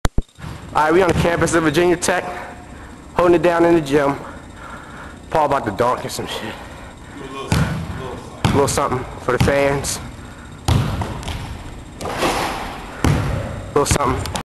Paul Dunking
Paul banging it with two hands at the VT practice court